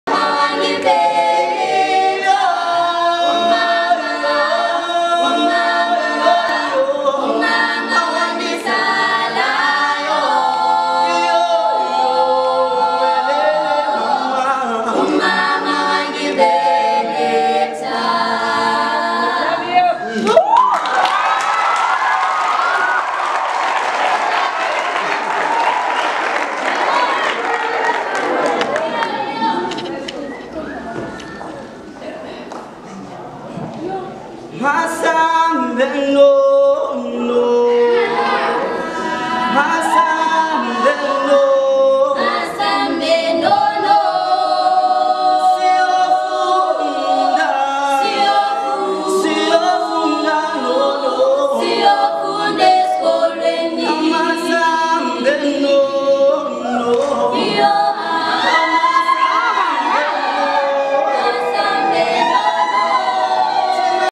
Communities and Justice Programme Award Ceremony in the North West Vaal Reef Technical School.
Learners of Vaal Reef Technical School and Matlhaleng Secondary School Celebrated the Certificate Award Ceremony